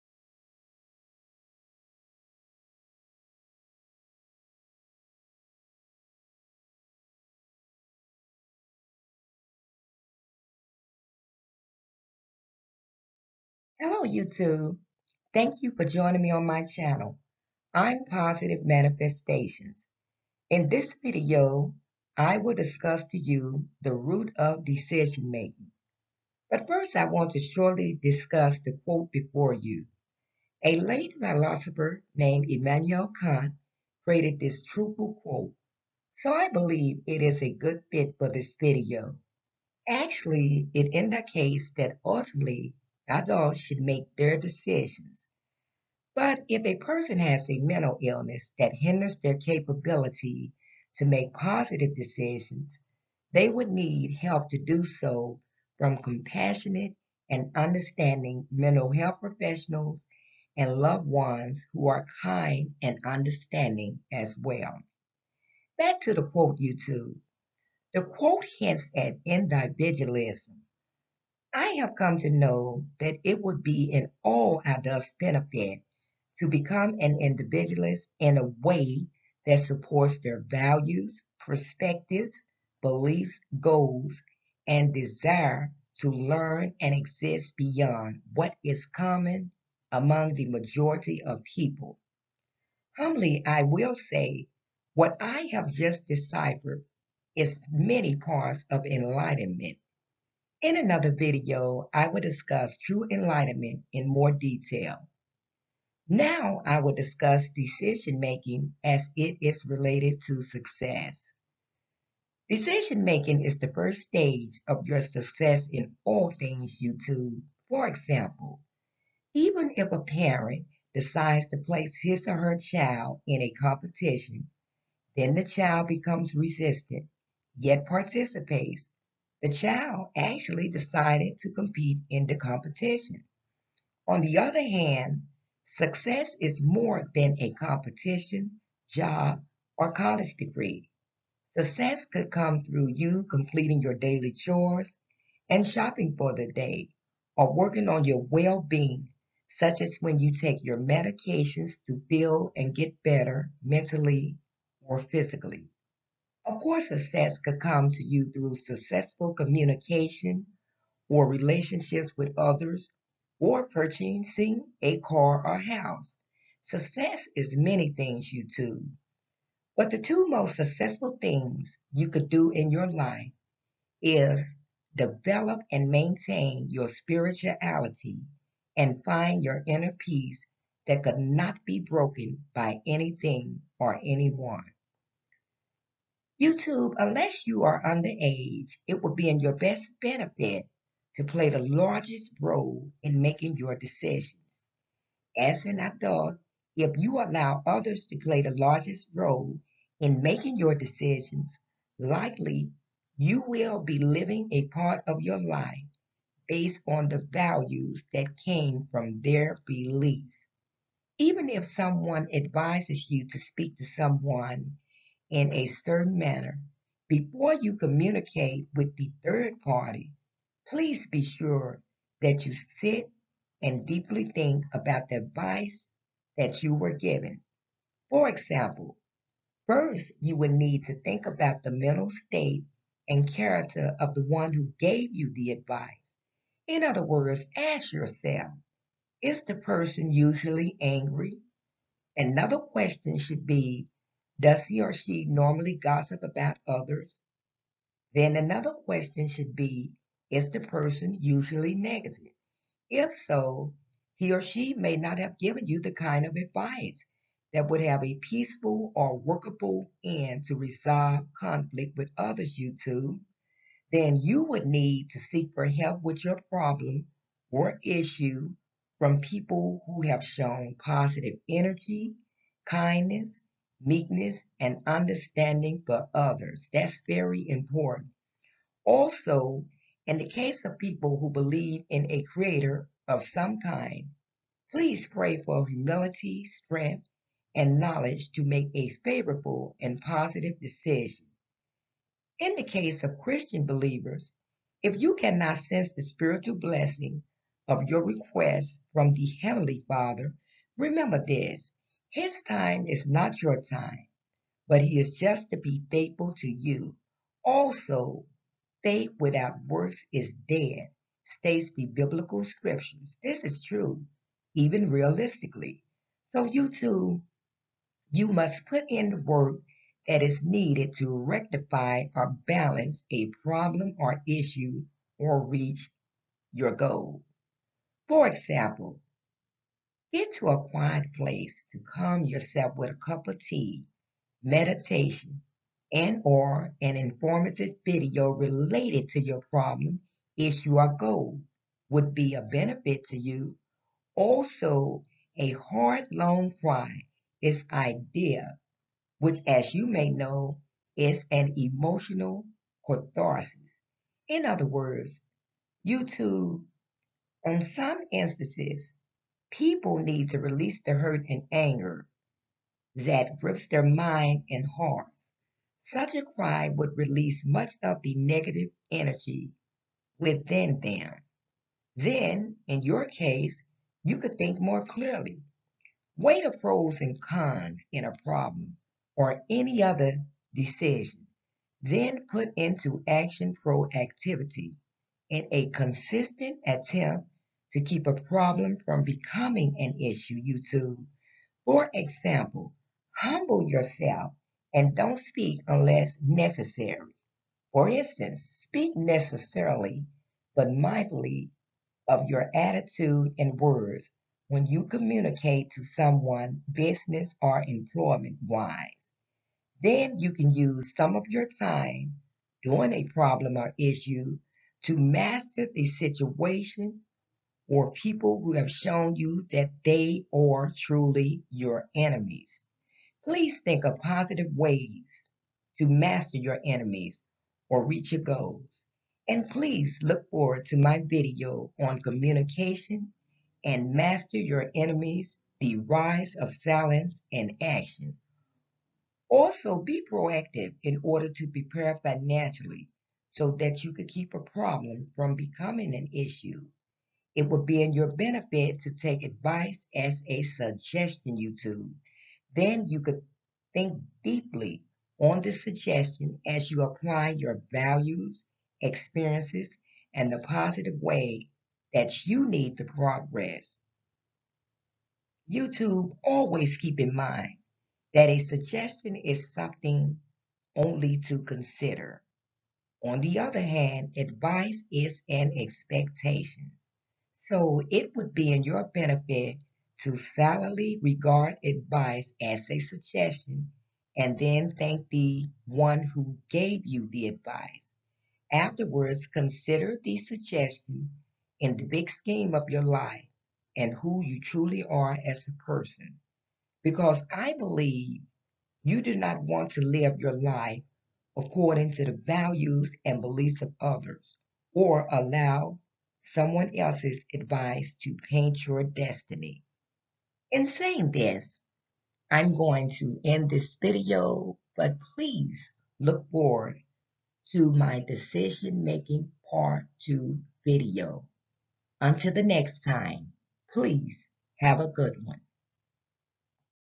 How to Make the Right Decisions: Start Your Success
In Decision Making 1, Positive Manifestations provides a detailed no-how on proper decision making as she empowers viewers to gain the courage to reach for their success in all things.